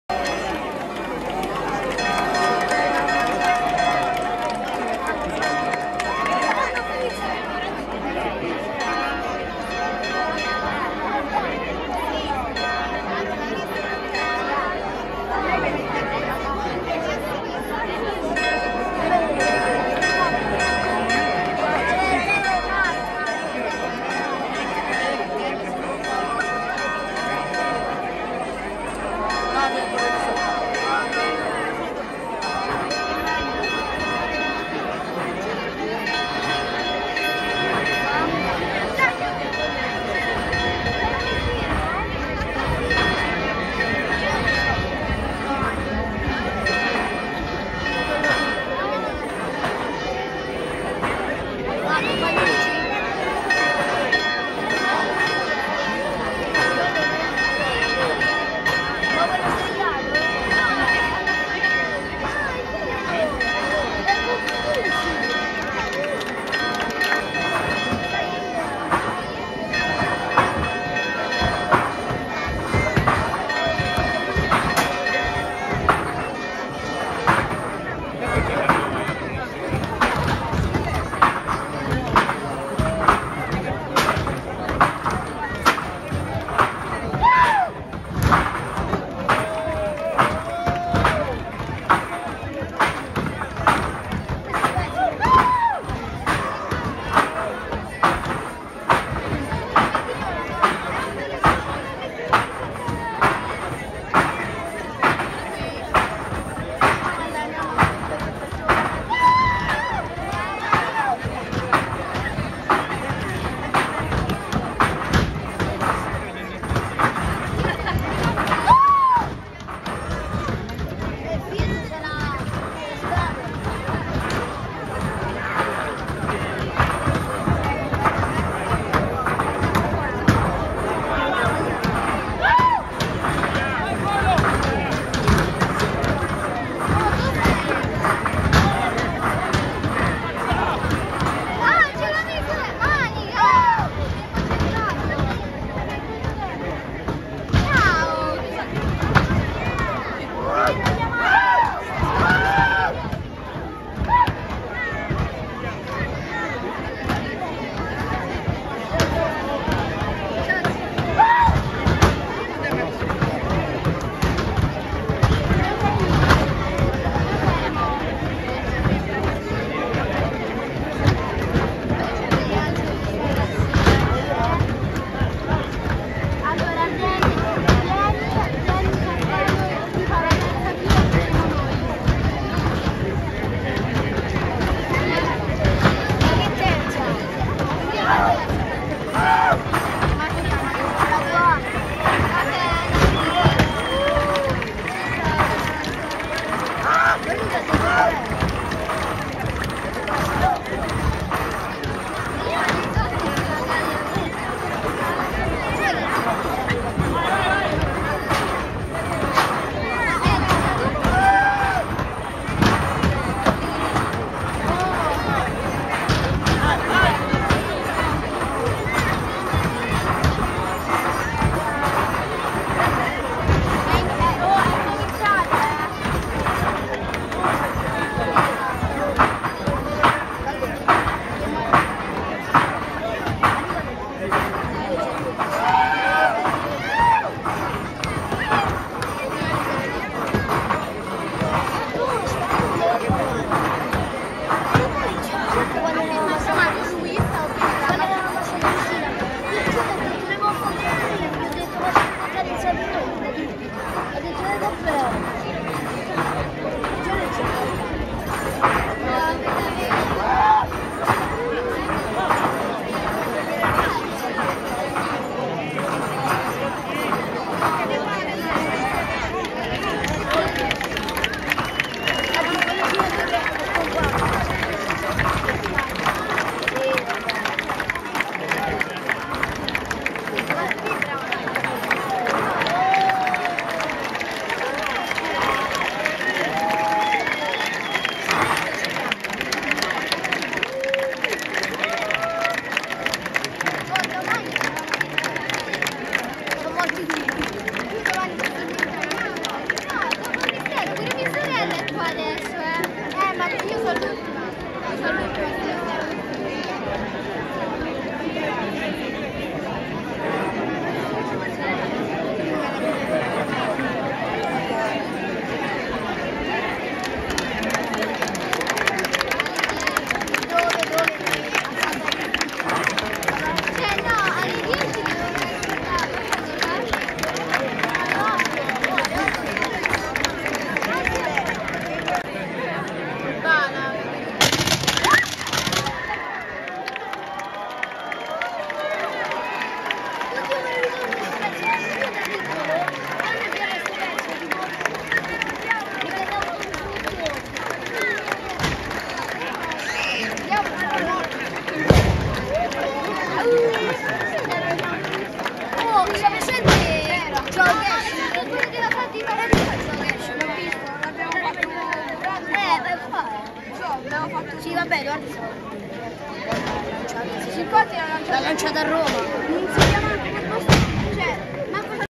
I Diavoli - Il Mistero di Santa Cristina a Bolsena (VT) 23 Luglio 2022 [ VIDEO INTEGRALE ]
Dopo 2 anni di STOP dovuto alla pandemia da COVID-19, sono andanti in scena i Misteri di Santa Scristina il 23 e il 24 Luglio 2022 a Bolsena. I MISTERI rappresentano i vari Martiri che ha subito la Santa protettrice di Bolsena. Nella notte del 23, la statua della Santa viene portata in processione per le vie del paese dalla Basilica di Santa Cristina fino alla chiesa del Castello. Durante il suo percorso, nelle piazze principali del paese vengono allestiti dei palchi dove i cittadini di Bolsena interpretano i Martiri della Santa. La mattina successiva, effettuando un percorso a ritroso, la statua della Santa ridiscende le vie del paese dalla chiesa del castello per rientrare nella Basilica a lei dedicata. Quello che vedremo nel video è uno dei Misteri più caratteristici di bolsena.. molto atteso dalle persone e che chiude la processione serale. Si tratta infatti del mistero dei Diavoli... buona visione!